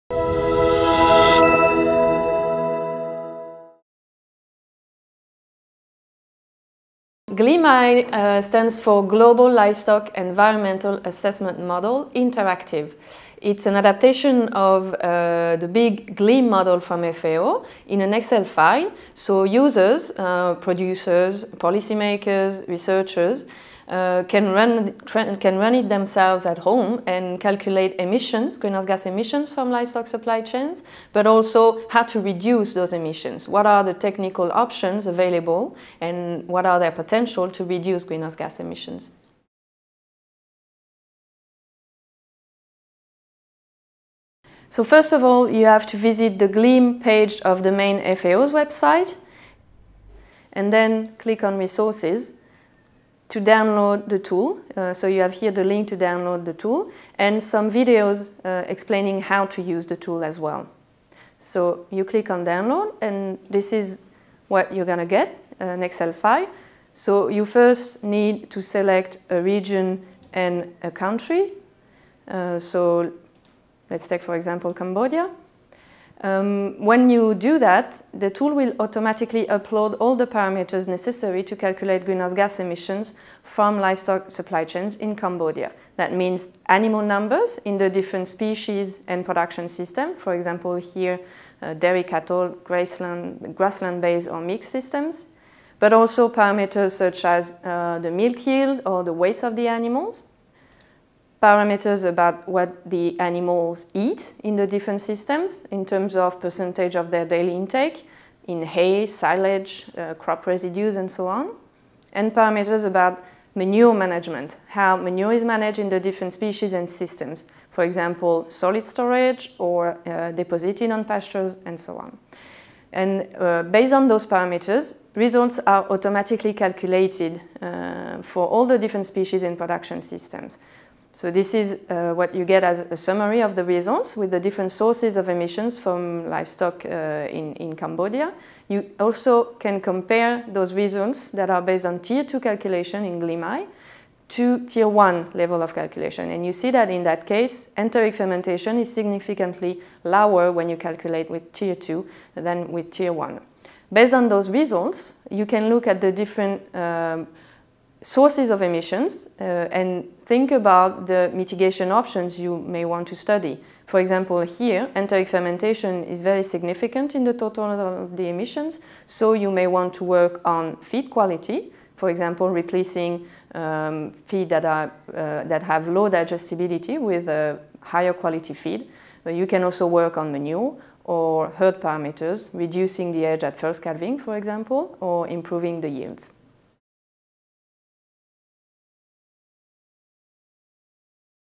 New tool from FAO helps reducing GHG emissions from livestock production
The GLEAM-i interactive tool allows users to simulate the greenhouse gasses emitted under different farming scenario. Anne Mottet, FAO Livestock policy officer, explains the advantages offered by this tool, designed to support governments, project planners, producers, industry and civil society. She then illustrates the single and user-friendly Excel file, available for download on the FAO website.